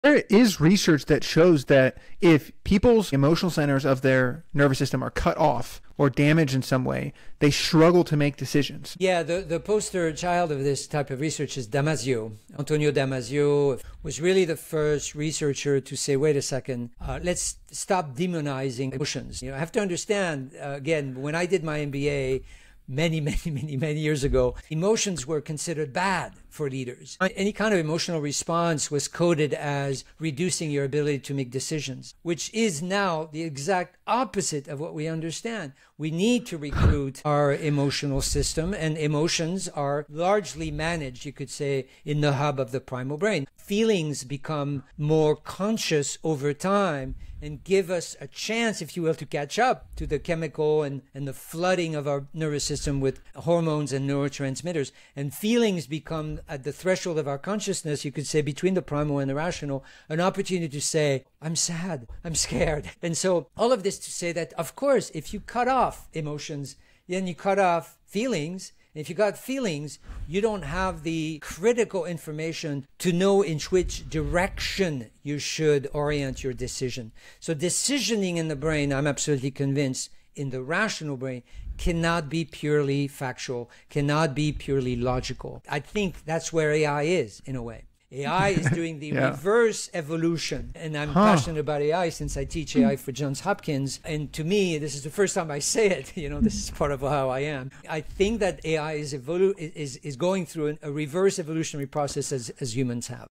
How Does Decision Making Work?
🧠✨ What if we told you that emotions are the most powerful marketing tool — not logic?

In this mind-expanding episode of The Decision Lab Podcast, neuromarketing pioneer Dr. Christophe Morin breaks down why the primal brain runs the show when it comes to decision-making. He shares research showing that when emotional centers in the brain are damaged, people can’t make decisions at all — proving that feelings aren’t distractions... they’re essential data.

For decades, marketers believed that emotion clouded judgment. But neuroscience now shows that emotion is the entry point to attention, memory, and action. Whether you’re building a brand or influencing behavior, if you’re not speaking to the emotional brain — you’re being ignored.

💥 Want to master persuasion? Start with understanding how emotions, hormones, and neurotransmitters drive behavior at a subconscious level.

🎧 Full interview with Dr. Morin — live now!